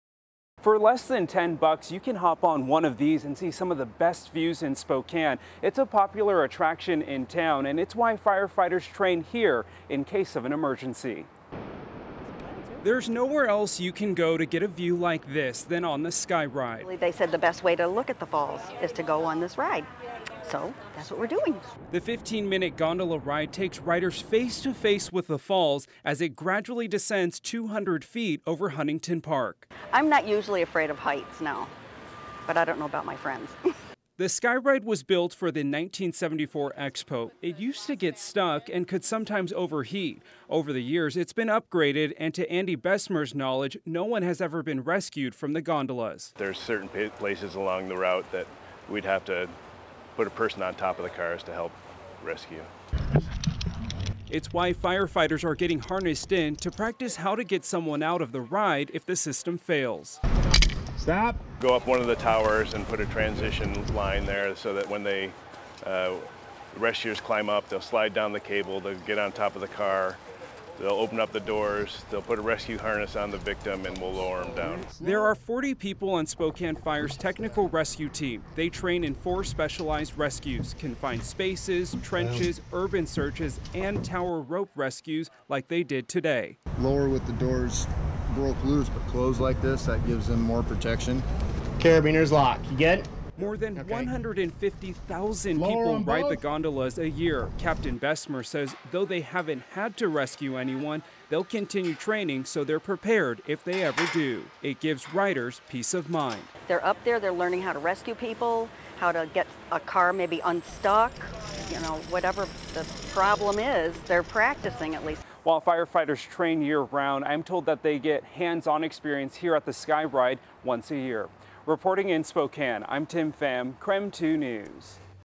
Spokane firefighters learn how to perform gondola rescues
Spokane Fire Department's Technical Rescue team trained on the SkyRide to get familiar with how to respond in the event of an emergency.